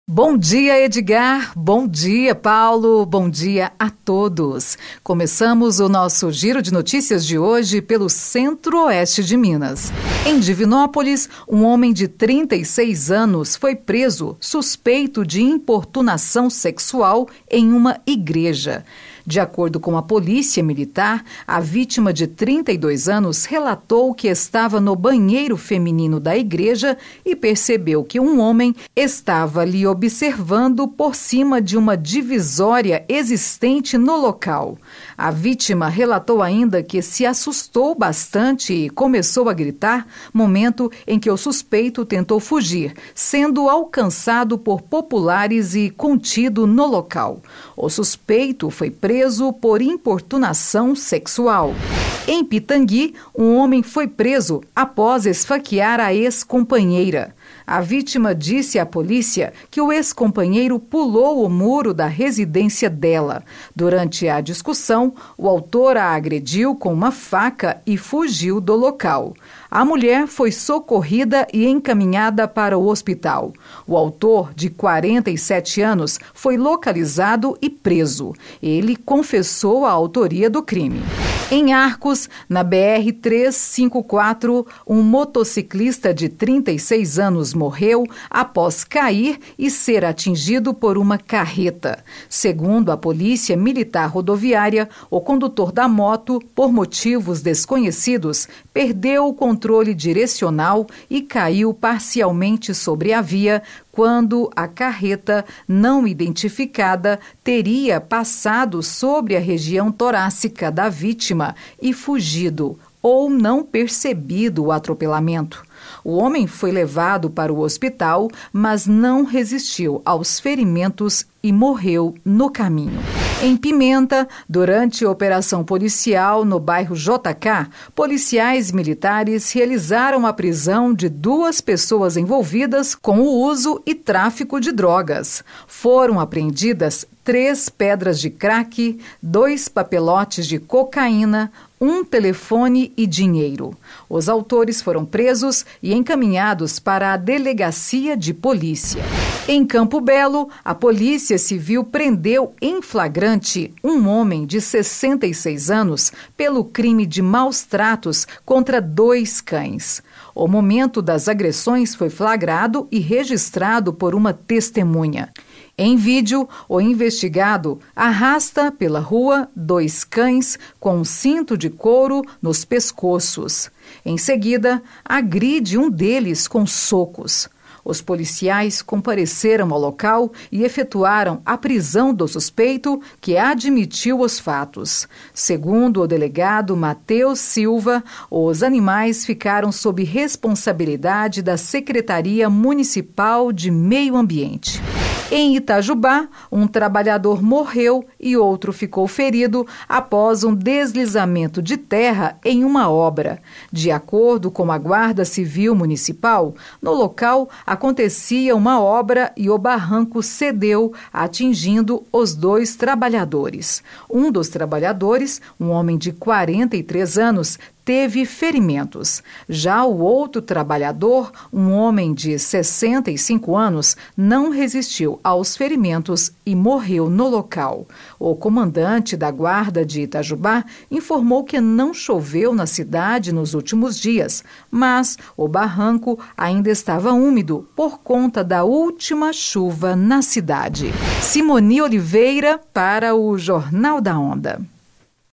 GIRO DE NOTÍCIAS 17/04/2024 - OS PRINCIPAIS DESTAQUES DAS REGIÕES CENTRO-OESTE E SUL DE MINAS GERAIS
Acompanhe os destaques:

I DIVINÓPOLIS I
HOMEM É PRESO POR IMPORTUNAÇÃO SEXUAL EM BANHEIRO DE IGREJA
I PITANGUI I
HOMEM É PRESO APÓS PULAR O MURO E ESFAQUEAR A EX-COMPANHEIRA
I ARCOS I
MOTOCICLISTA MORRE ATROPELADO POR CARRETA NAS MARGENS DA BR-354
I PIMENTA I
DUAS PESSOAS SÃO PRESAS POR TRÁFICO DE DROGAS
I CAMPO BELO I
POLÍCIA CIVIL PRENDE SUSPEITO DE MALTRATAR CÃES EM CAMPO BELO
I ITAJUBÁ I
TRABALHADOR MORRE E OUTRO FICA FERIDO APÓS DESLIZAMENTO DE TERRA EM OBRA